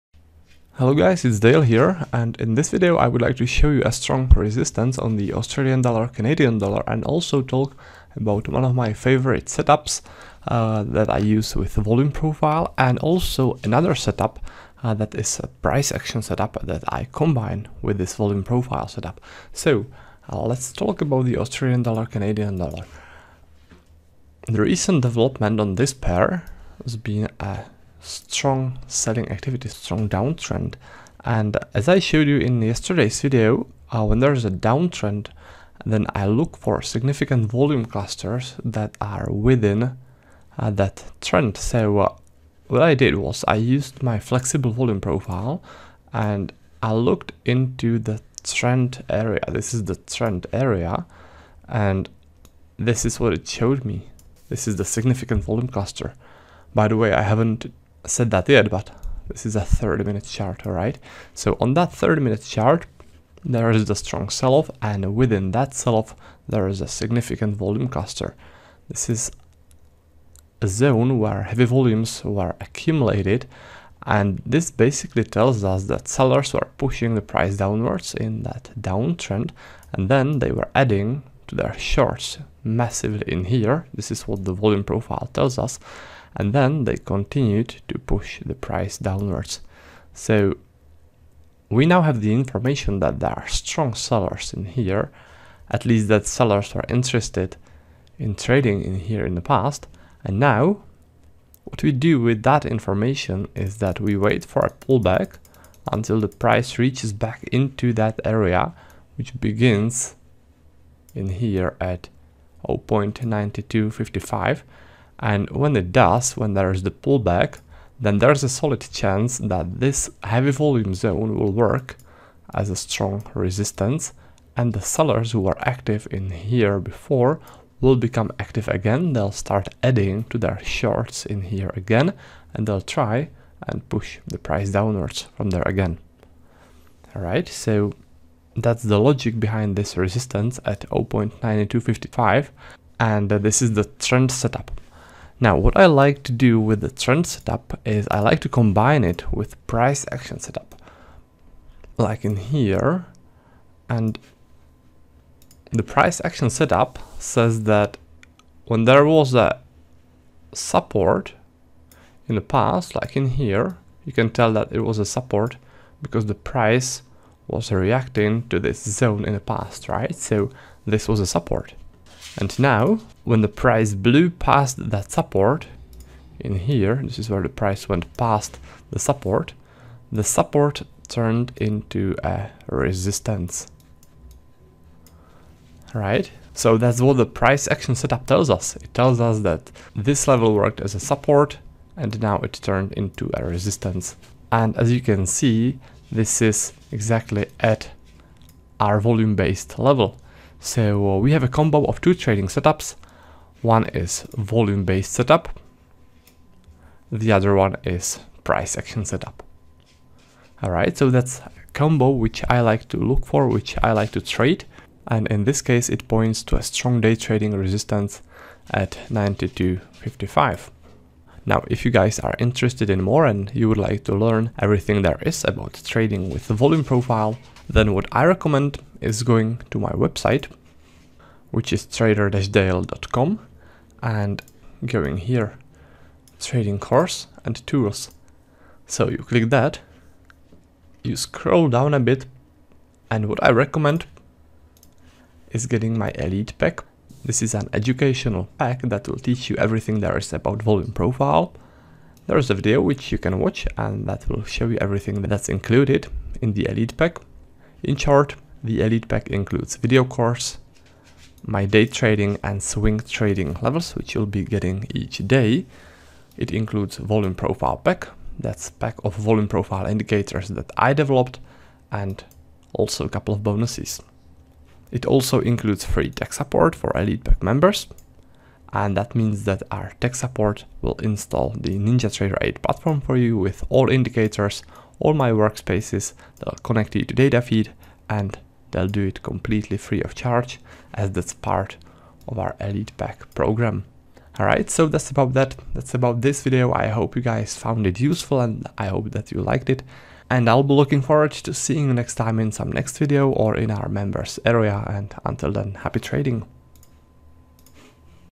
Volume Profile & Price Action Setup Combo On AUD/CAD
CONTENT
intro 0:00
Volume Profile Setup 0:40
Price Action Setup 2:27
Further Education 3:40

Happy trading!
-Dale